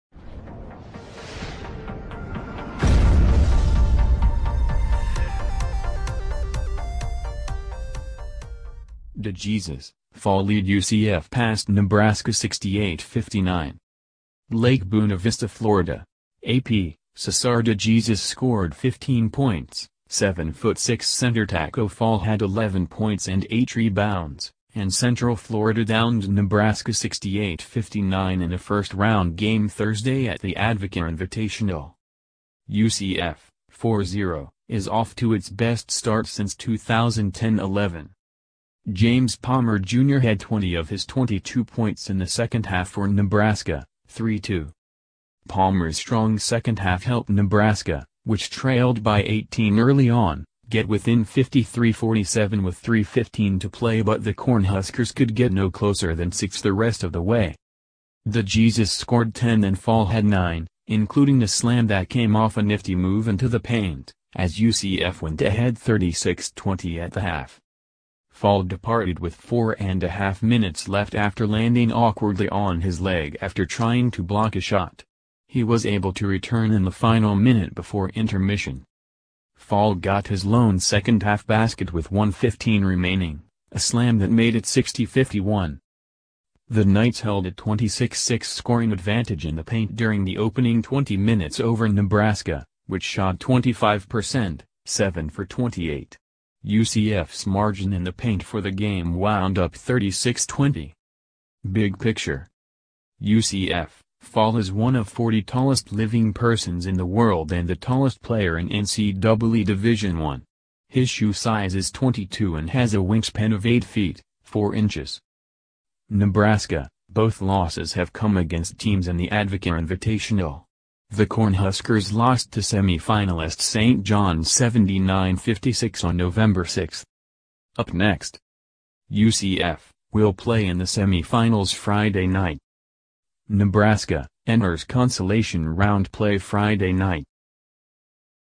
DeJesus, Fall lead UCF past Nebraska 68 59
LAKE BUENA VISTA, Fla. (AP) — Ceasar DeJesus scored 15 points, 7-foot-6 center Tacko Fall had 11 points and eight rebounds, and Central Florida downed Nebraska 68-59 in a first-round game Thursday at the Advocare Invitational.

UCF (4-0) is off to its best start since 2010-11

James Palmer Jr. had 20 of his 22 points in the second half for Nebraska (3-2).